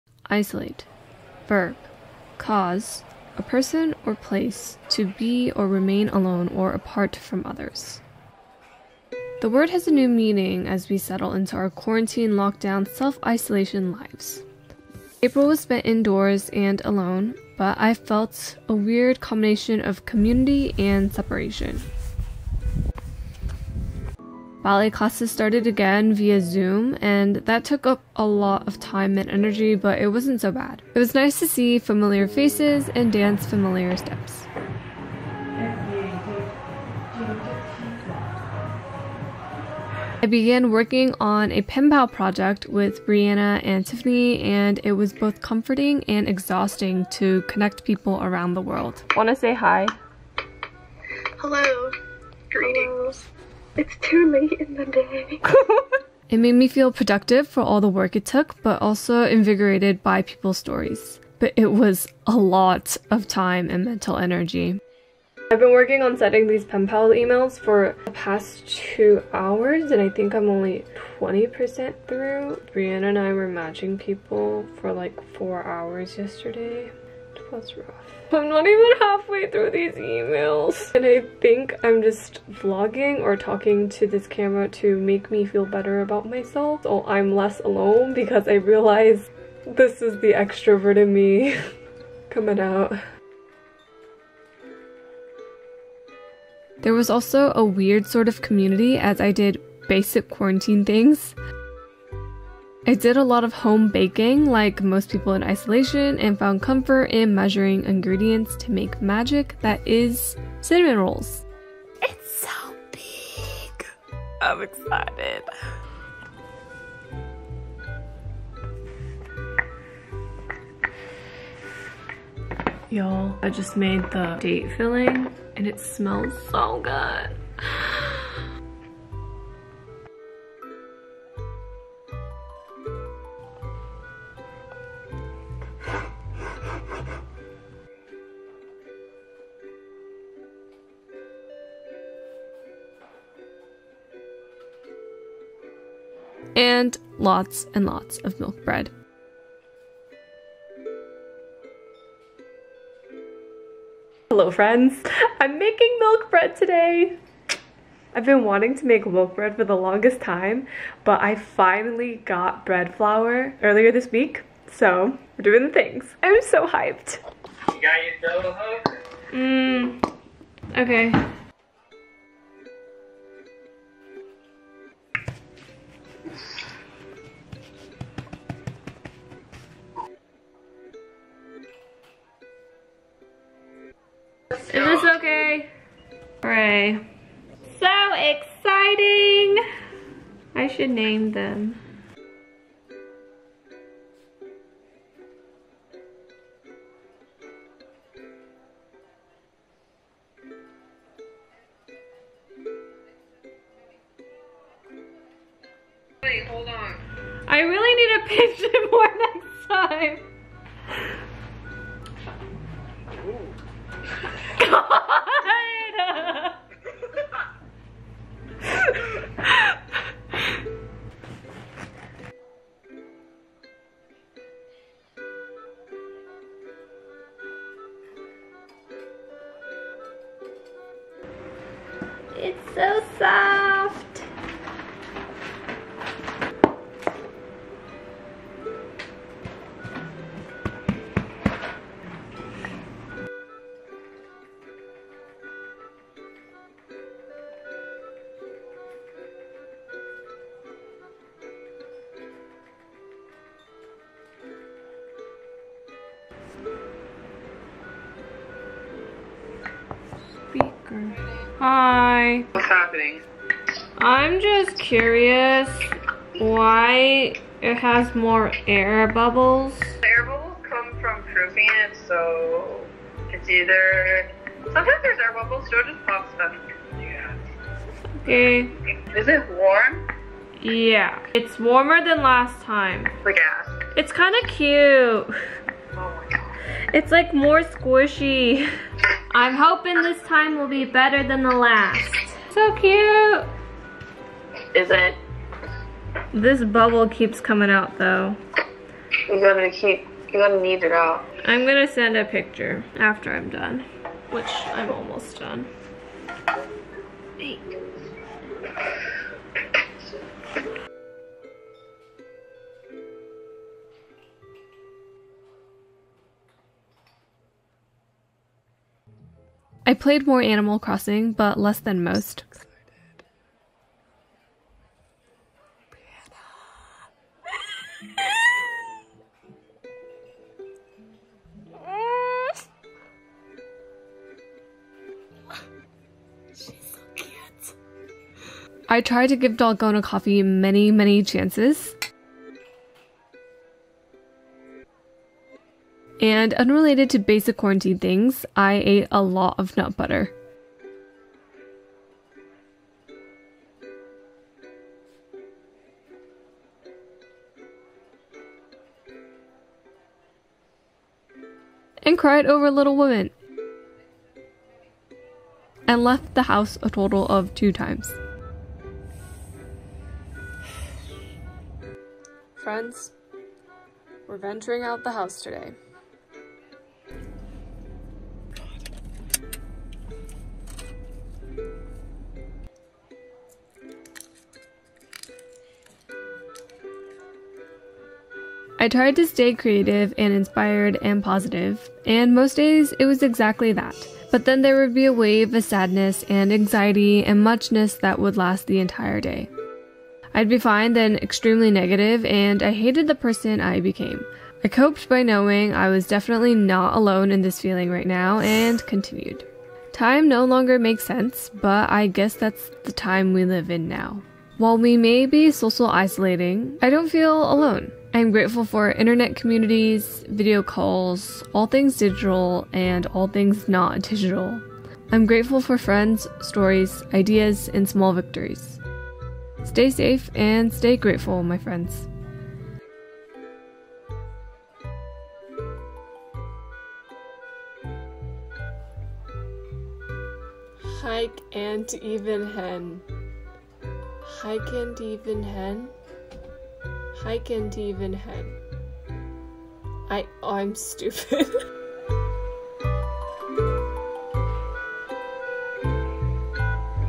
a month in isolation。visual diary & vlog
📮 open for a letter & links  🔗

dear friend,

it’s been a very long month, but we made it! even though (or maybe because??) my environment didn’t change, my mental state and mood was VERY inconsistent. i’d feel really motivated one day and low energy the next. quarantine never felt like a *big thing* until randomly i’d get down on small things. like going out to eat. or taking a class. or the idea of traveling in general, near or far, without extra fear.

anyway, i hope you had a good month; if not, know there’s another one coming which is another opportunity for better things.

xox alexandra